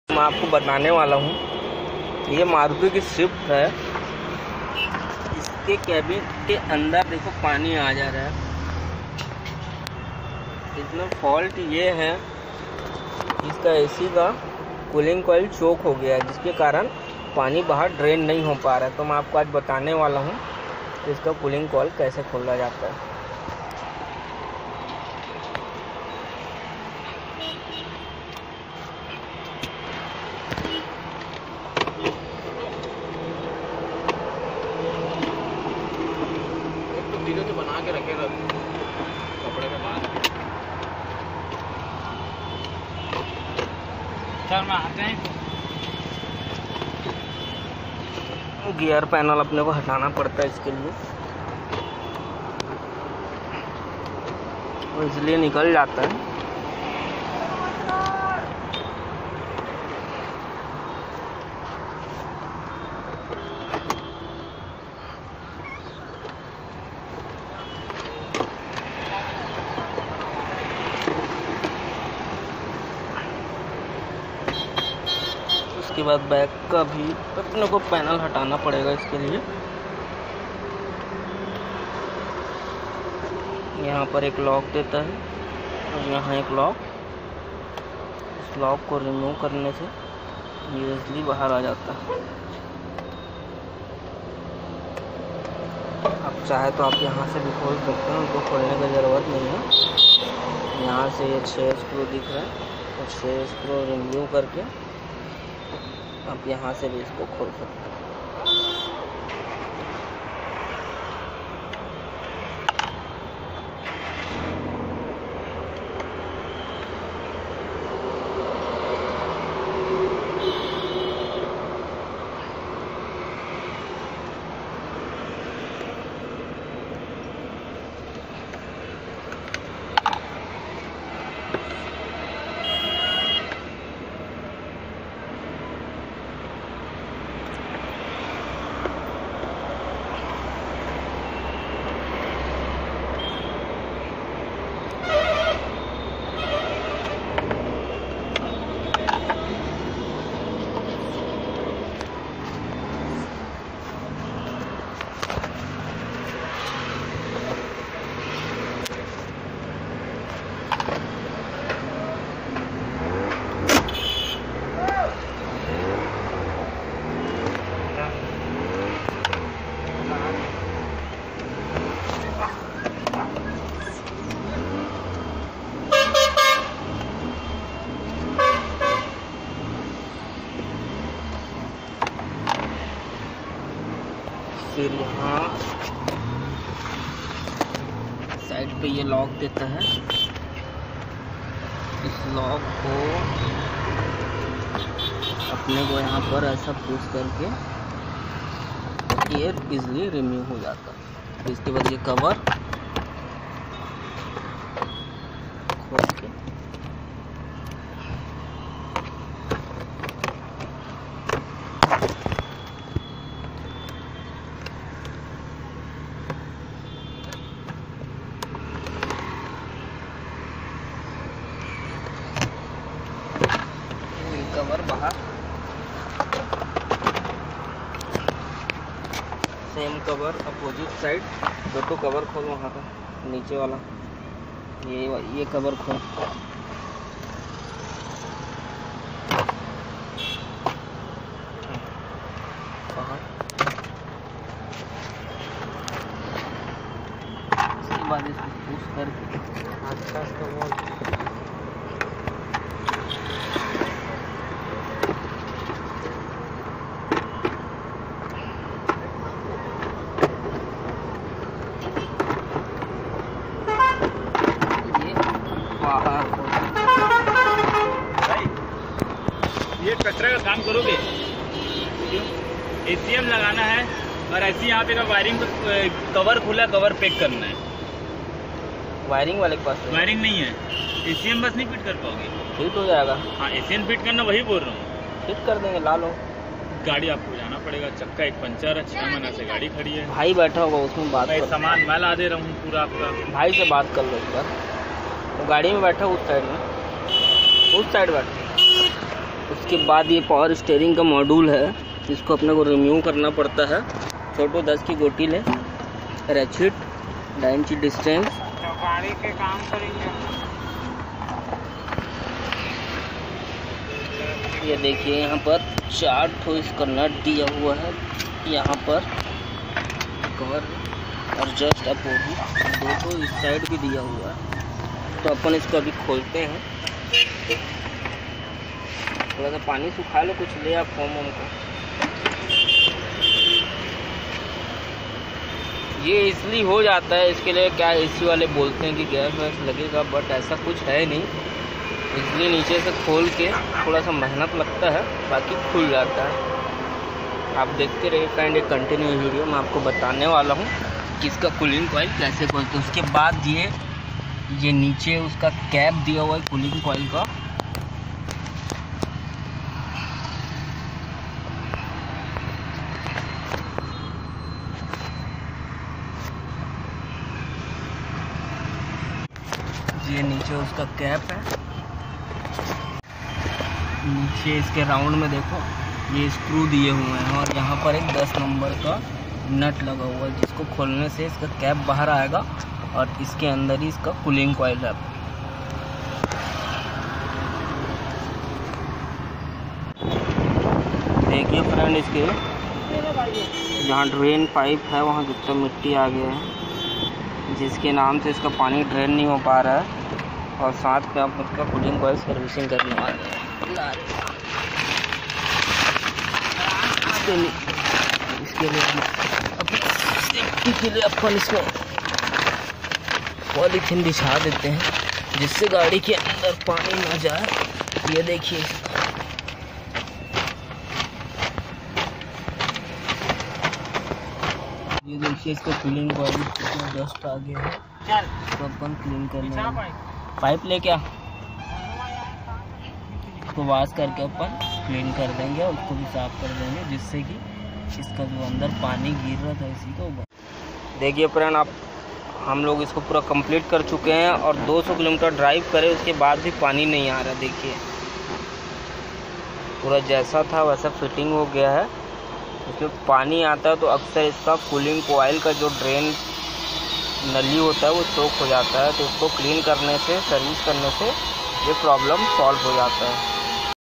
Maruti swift cabin water problem fault in ac
maruti swift cabin water problems in ac